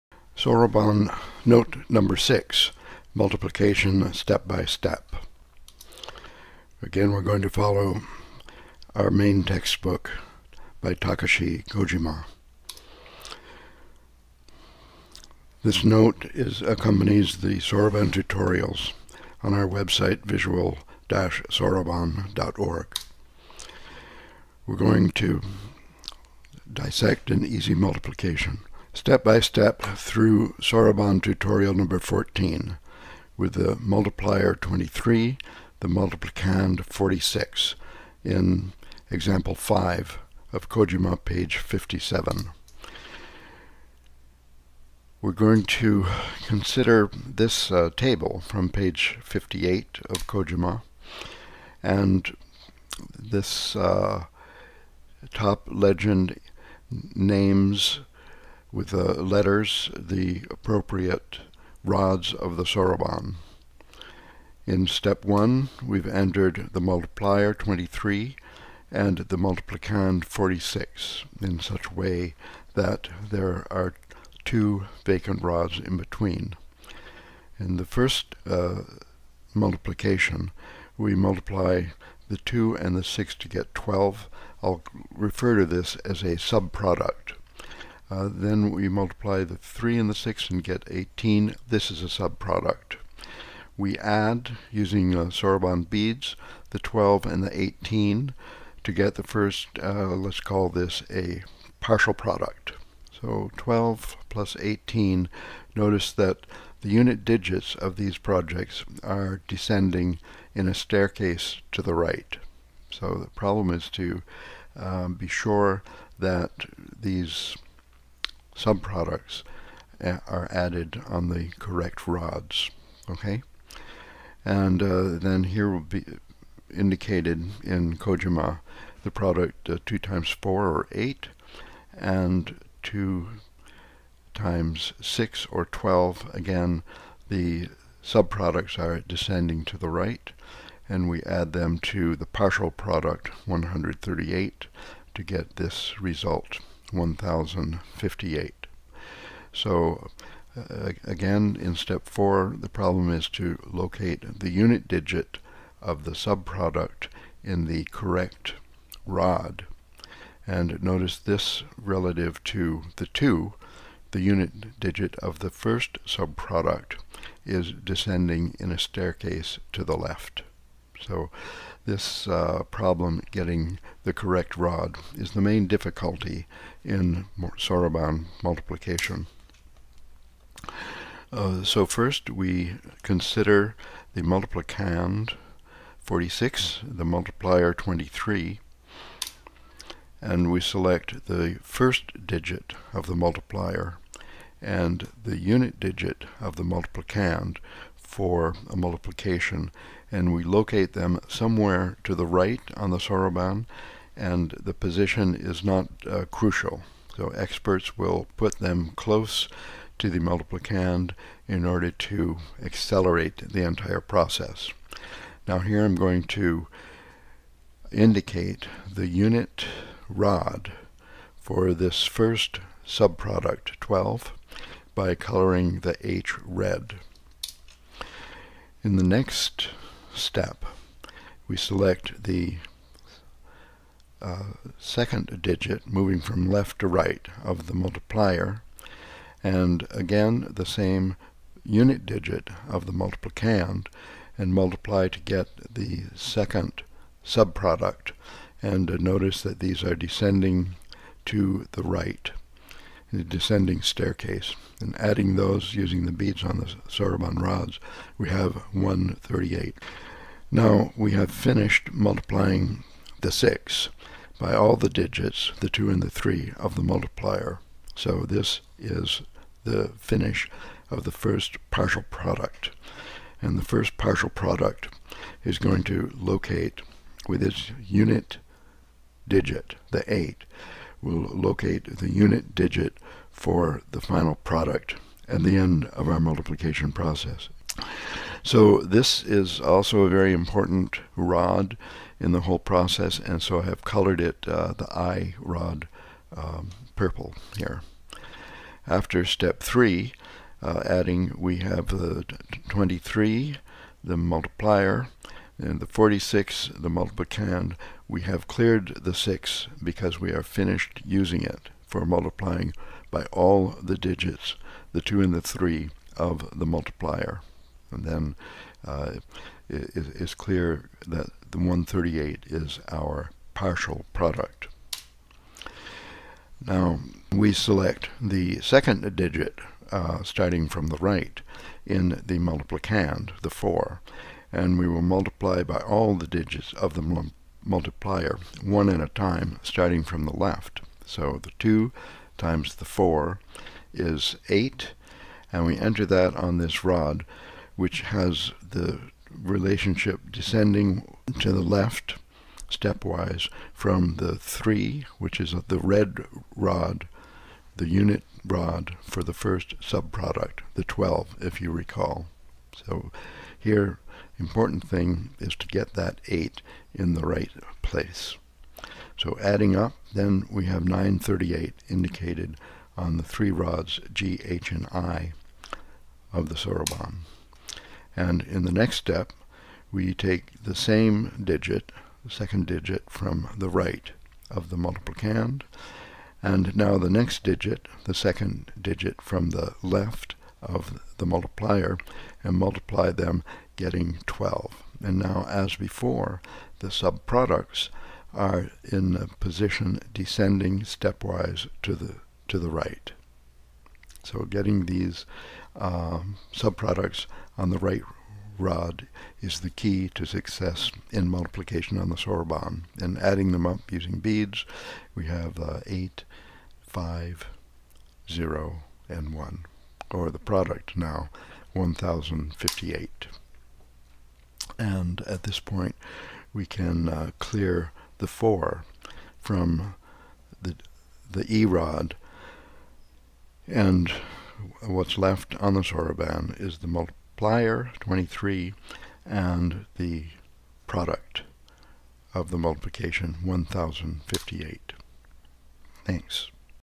Soroban Note #6, Multiplication Step-by-Step
Dissection of a soroban multiplication by the two-digit multiplier shown in Soroban Tutorial #14. By Ralph Abraham, Visual Math Institute. visual-soroban.org.